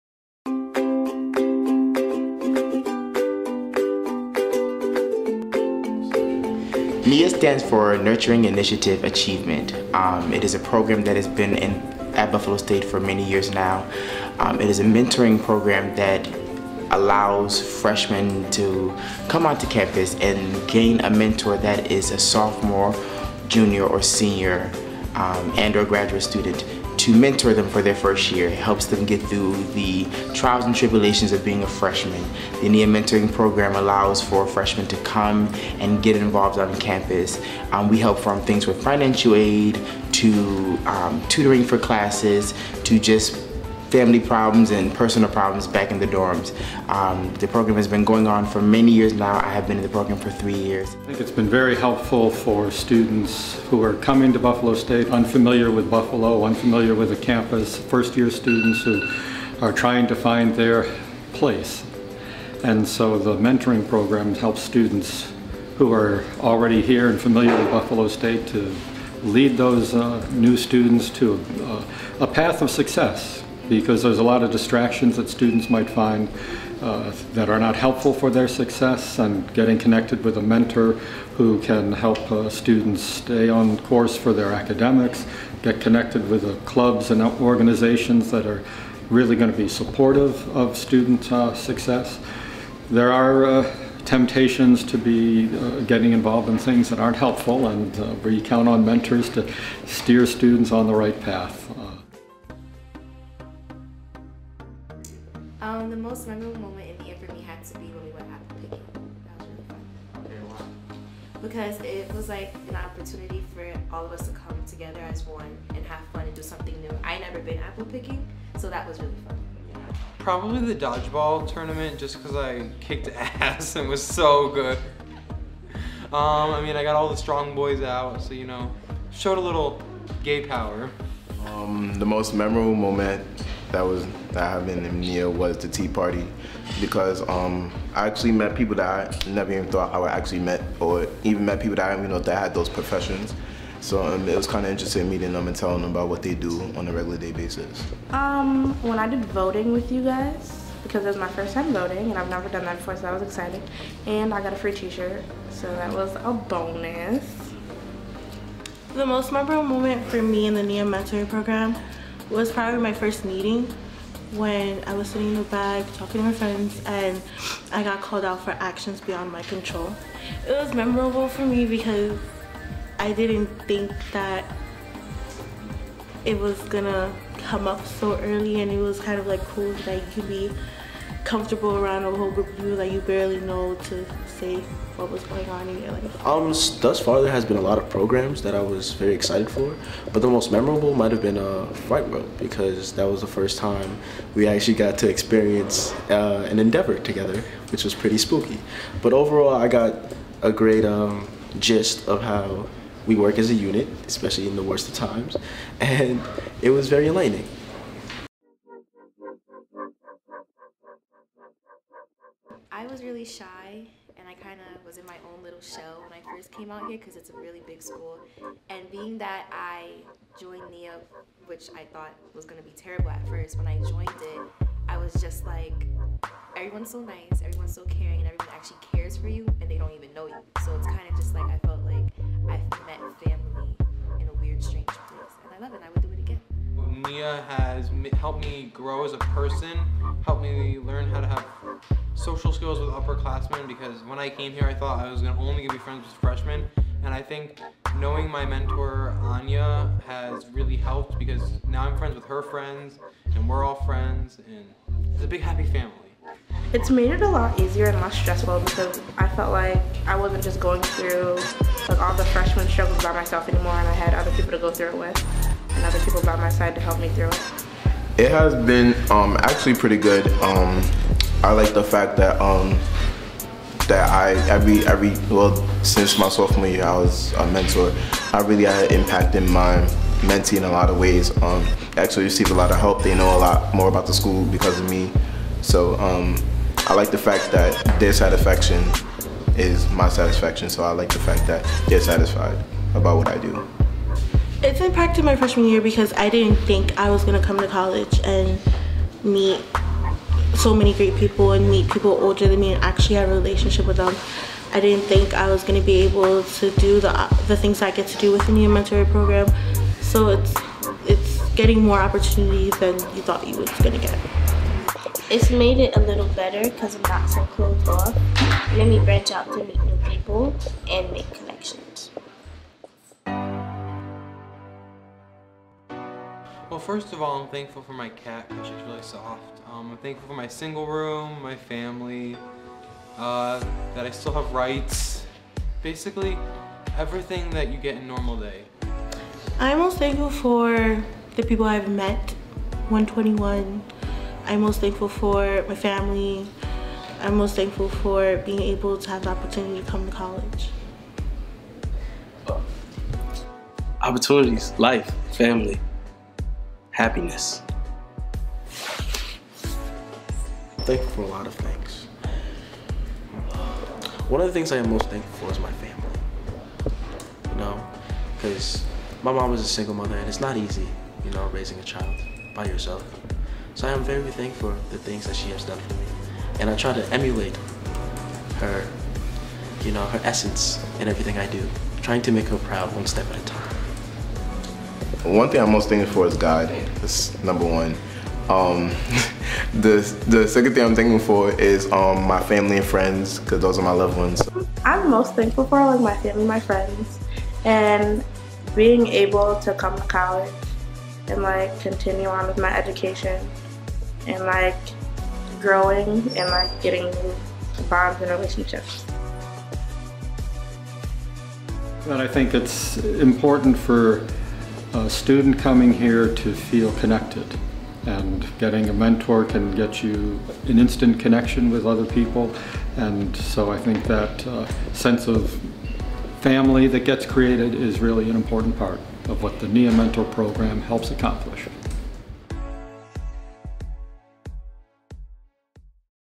NIA MENTORING PROGRAM
Students and Faculty discuss how the NIA MENTORING program at Buffalo State College impacted them .